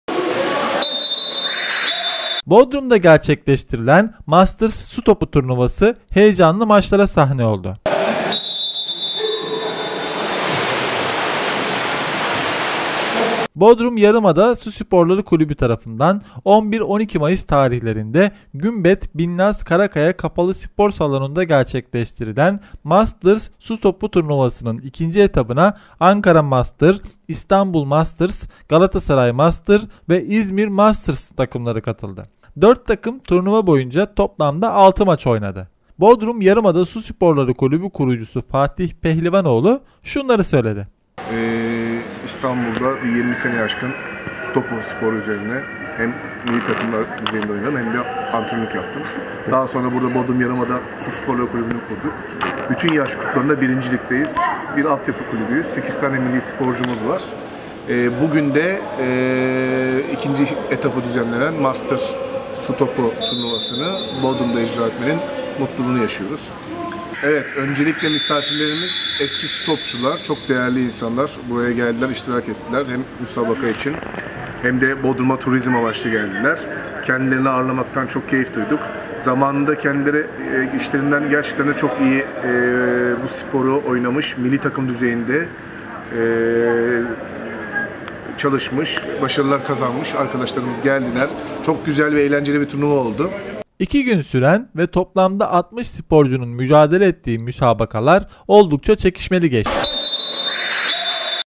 HAVUZDA HEYECAN VARDI
Bodrum’da gerçekleştirilen “Masters Su Topu Turnuvası” heyecanlı maçlara sahne oldu.